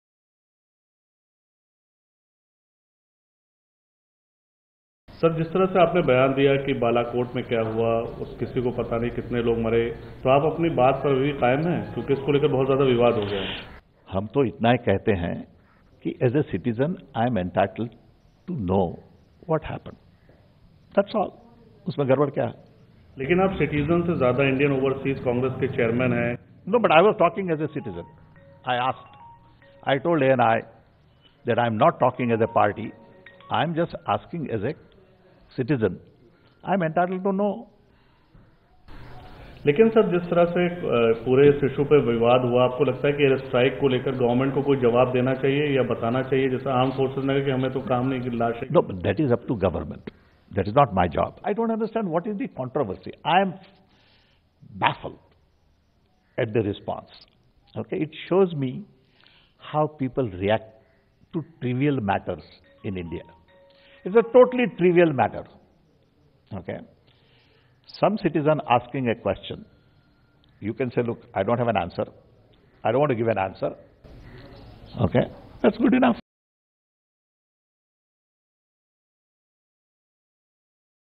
Not talking on party’s behalf, speaking as citizen: Sam Pitroda on his Balakot air strike remark
New Delhi, Mar 22 (ANI): Chairman of Indian Overseas Congress Sam Pitroda who sparked a controversy with his comment on Balakot air strike, clarified his stance.